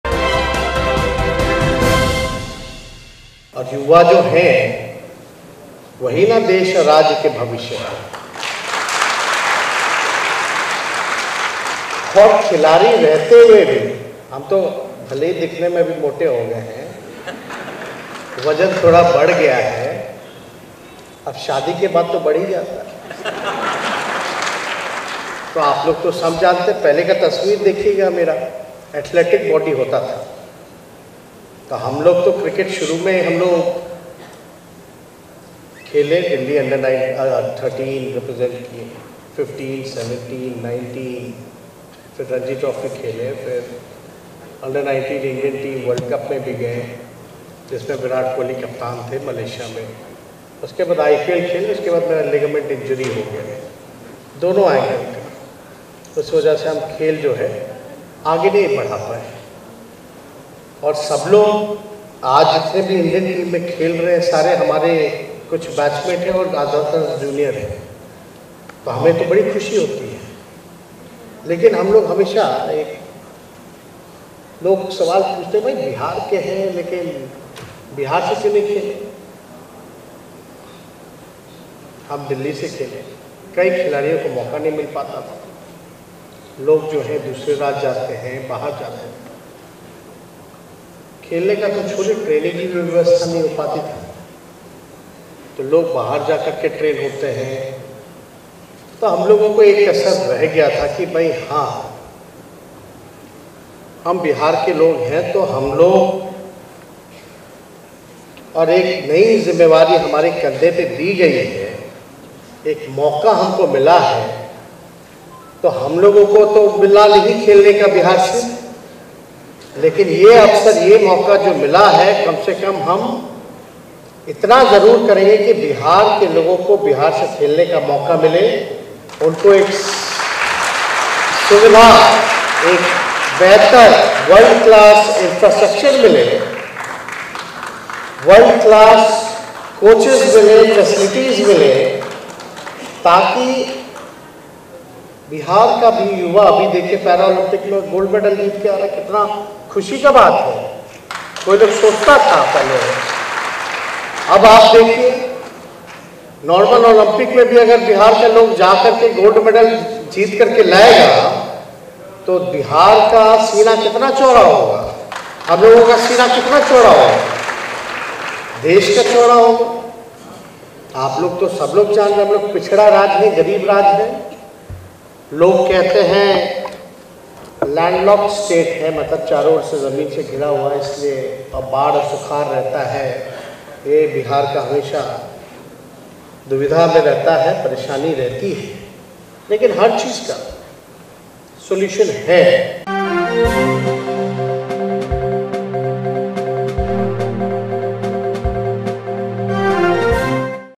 शादी के बाद हम भी थोड़ा मोटा हो गए हैं भरे मंच से Tejashwi Yadav ने लूट ली महफिल
शादी के बाद हम भी थोड़ा मोटा हो गए हैं भरे मंच से Tejashwi Yadavने लूट ली महफिल